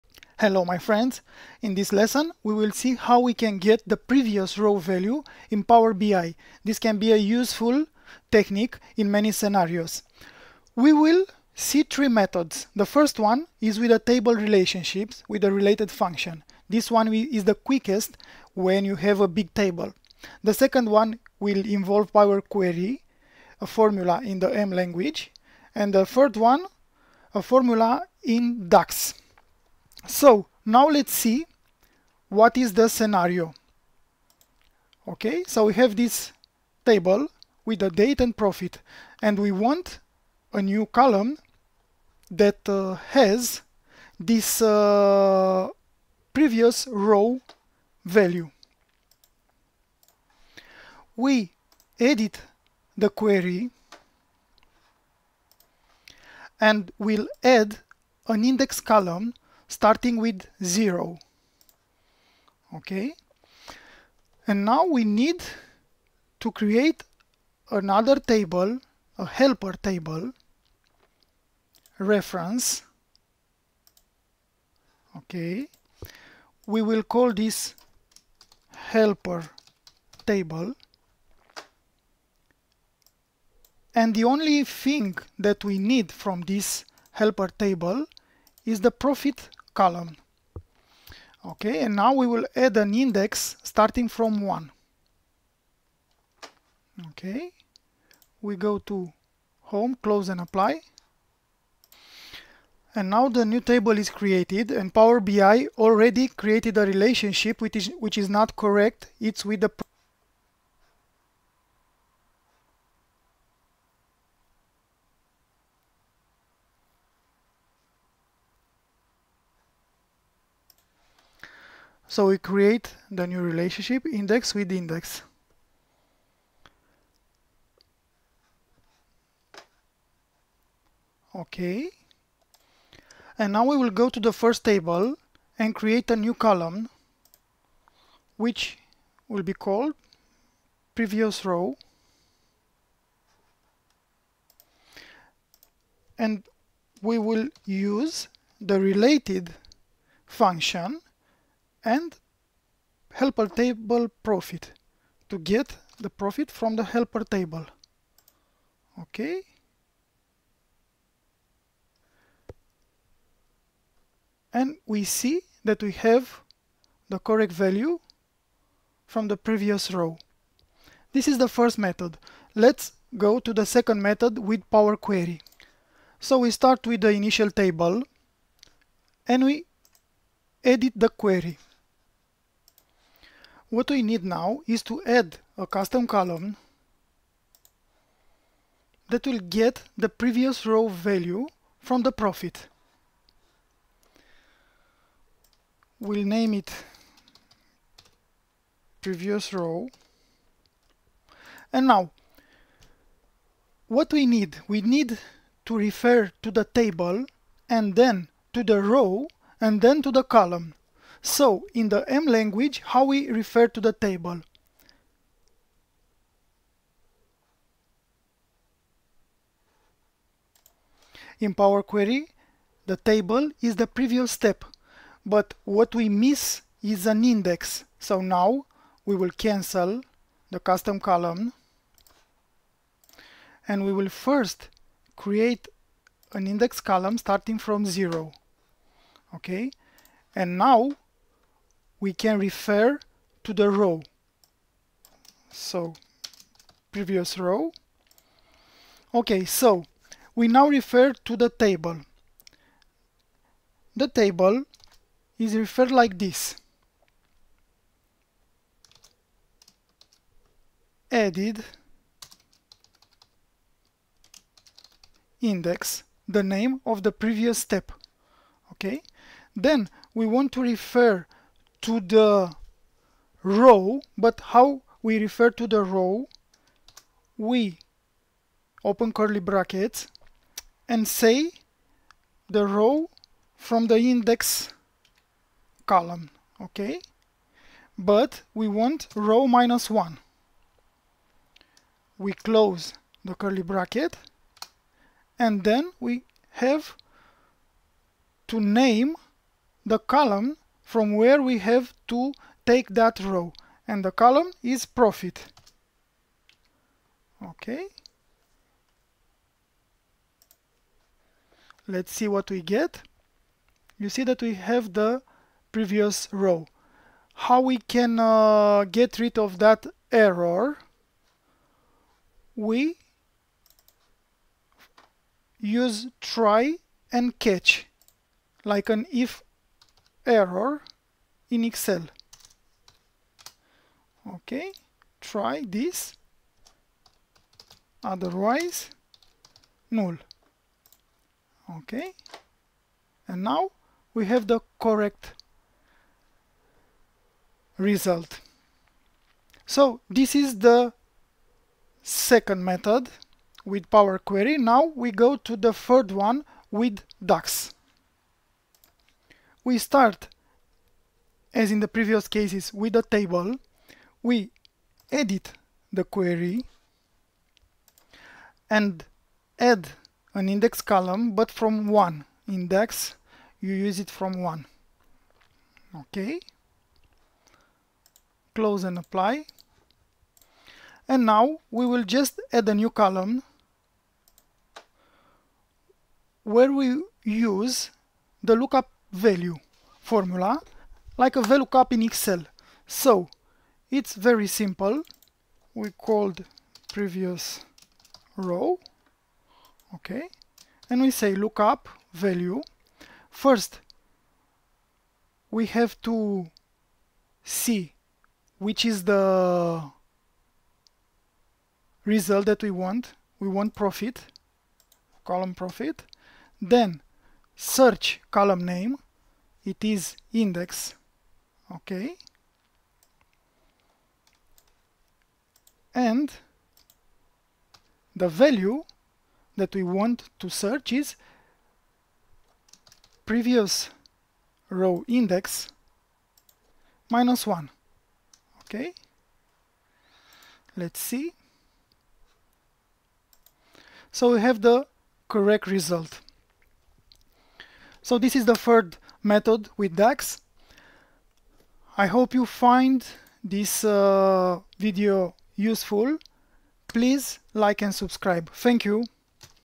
Get Previous Row Value in Power BI
3 Methods for getting the previous Row Value in Power BI and Power Quey:

- Tables Relationship (RELATED function)
- Power Query (M Language formula)
- DAX (LOOKUPVALUE function)